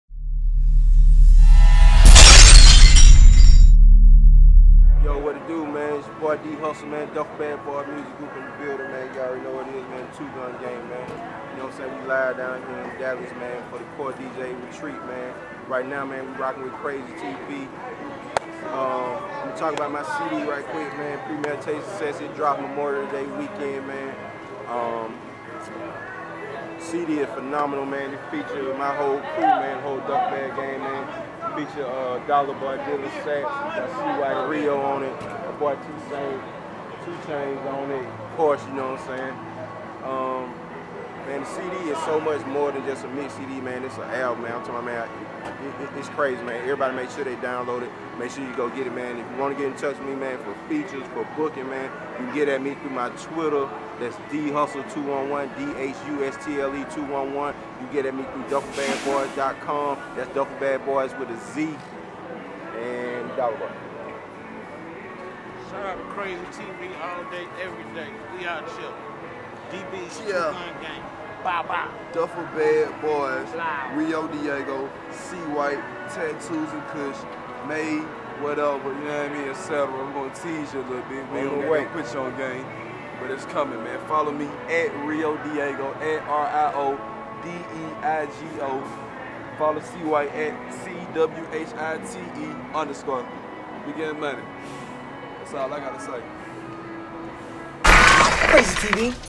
2 Chains Before He Got Big
MixShowlive 2012 Dallas Texas
Playa Circle with D Hustle, Dolla Boy, 2 Chains, C White, Rio Diego
before 2 chains took off and made it big